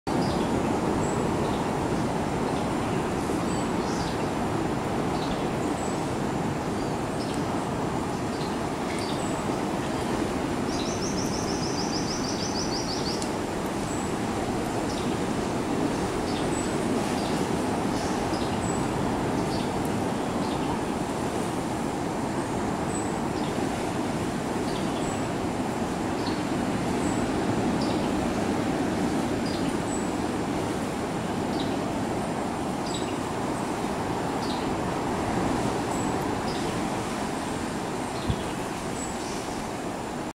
Spider monkey swinging through the tree with his tail
Spider monkey swinging through the tree with his tail. Somewhere on the Osa Peninsula, Costa Rica